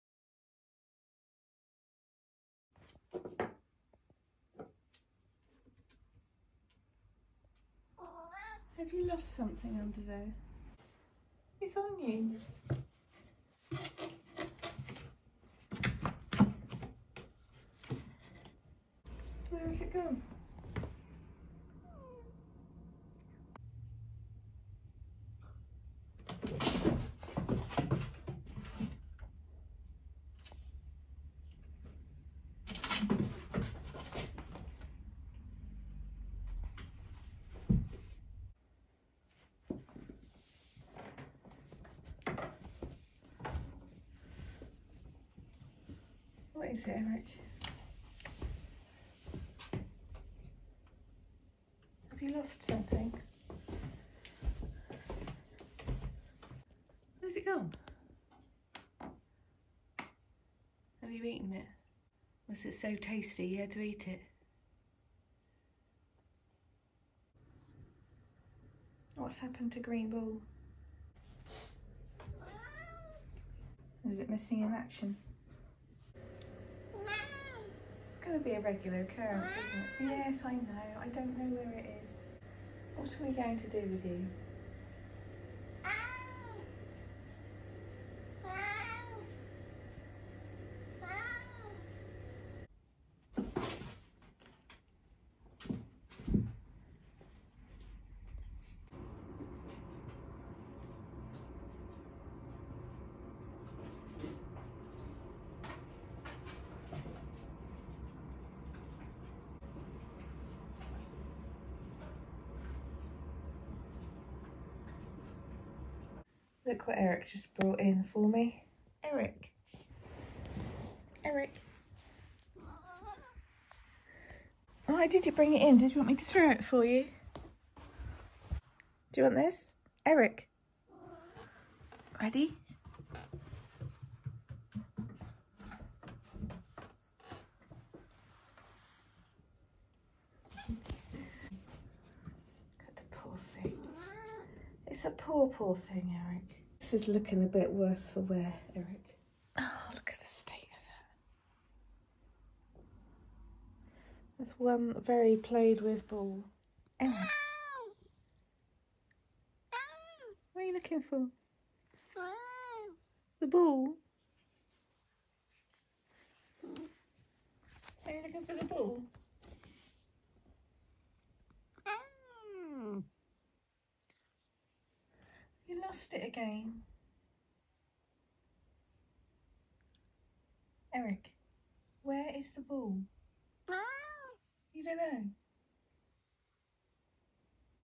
Eric: a love story (involving a green ball)
Eric has been through a turbulent time since this little green ball came into his life three weeks ago.
It’s been lost, found and lost again. But his love for it has never stopped.

Eric can be found chasing his ball every day, he is also known for talking way more than any cat should.
He is 12 years old and lives in Britain.